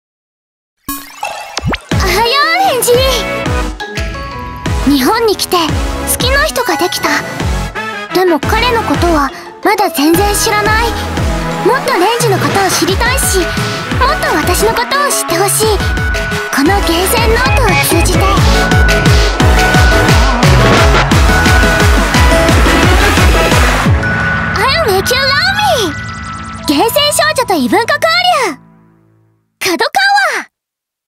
英国少女リリーを東山奈央が熱演！漫画『ゲーセン少女と異文化交流』PV
英国生まれのNEWヒロイン その名はLily

「次にくるマンガ大賞2021」Webマンガ部門　ノミネート！
英国少女とゲーセン店員が繰り広げる ザ・ラブコメ
『ゲーセン少女と異文化交流』

コミックス2巻発売を記念し、リリーの愛らしさがギュッと詰まったPVが完成！

【出演】
リリー：東山奈央

Twitter発！ キュートな英国少女とのほのぼのコメディ
ゲーセンでバイト中の主人公・草壁蓮司は、ひょんなことから英国少女のリリーと出会い、
なぜだか交換日記をする仲に!?
天真爛漫なリリーに振り回される蓮司だが、果たしてこの異文化交流は友情か、それとも……？

著     / 安原宏和
発行   / 株式会社ＫＡＤＯＫＡＷＡ ドラゴンコミックスエイジ
発売日 / 第2巻 2021年7月9日